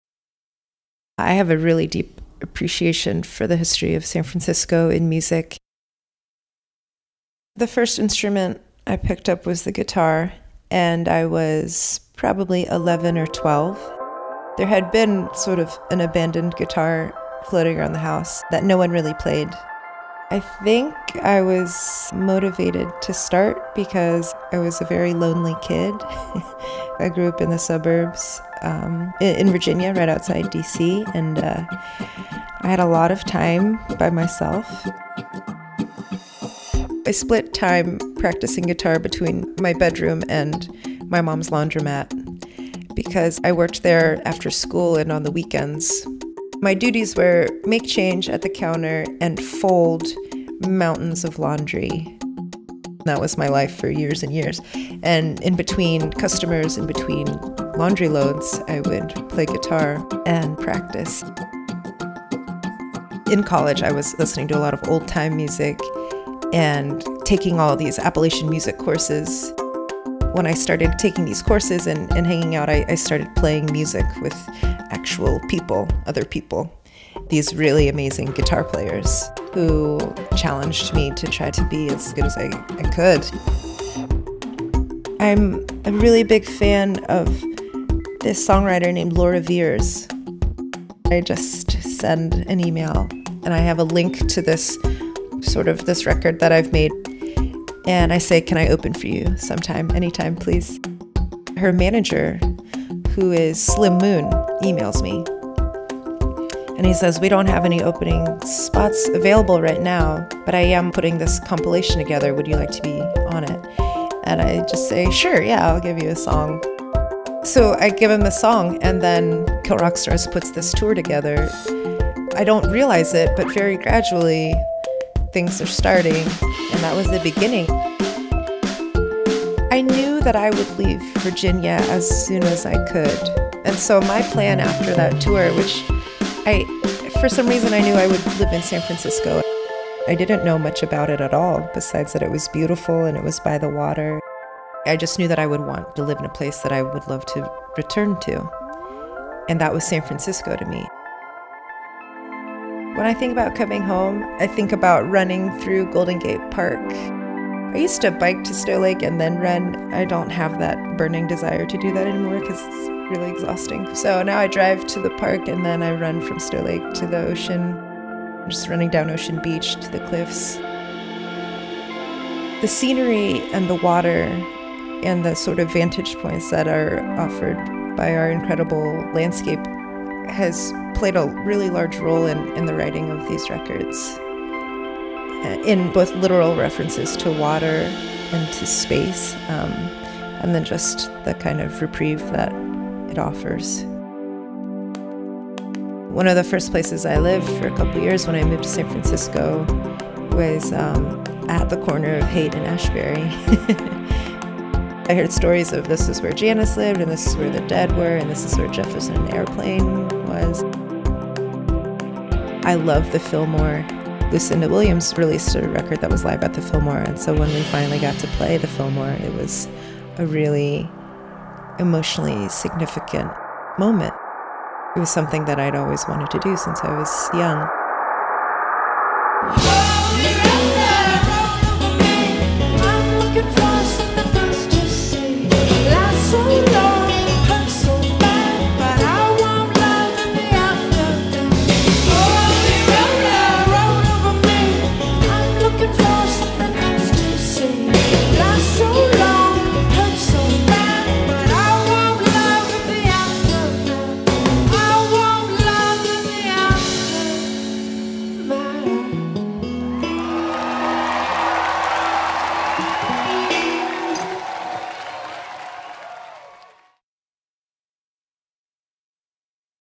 California Inspires Me: Thao Nguyen
Singer/songwriter Thao Nguyen speaks on learning to play guitar at her mom’s Laundromat, her rise to success, and what she loves about San Francisco in California Inspires Me, a Google Play x California Sunday Magazine collaboration.